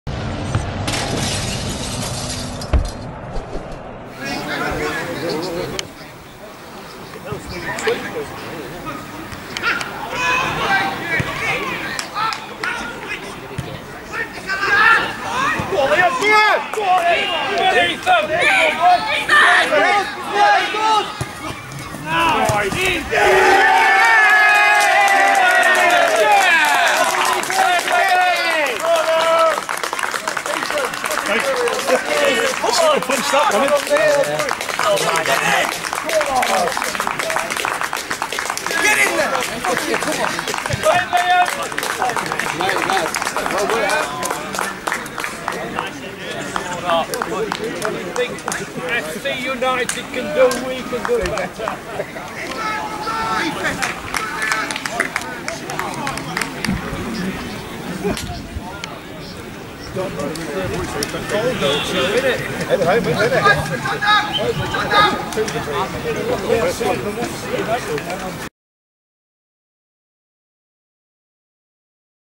Liam Waldock adds the third | Marske - 18/10/22
Four minutes into stoppage time, Waldock releases Ethan Bojang down the left, the Gambia U20 international draws the 'keeper and squares to complete the one-two and put the icing on the cake for Trinity!